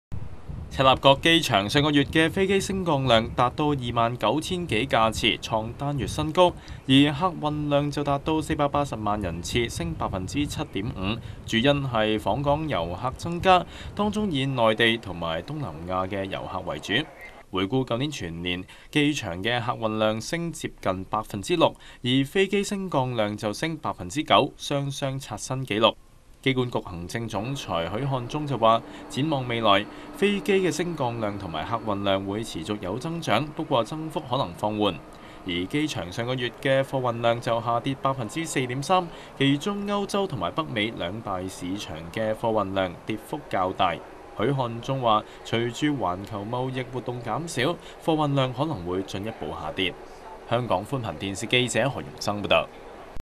機場客運量再創新高
本港機場去年全年的客運量和飛機升降量都刷新紀錄，機管局行政總裁許漢忠說，未來客運量會持續有增長。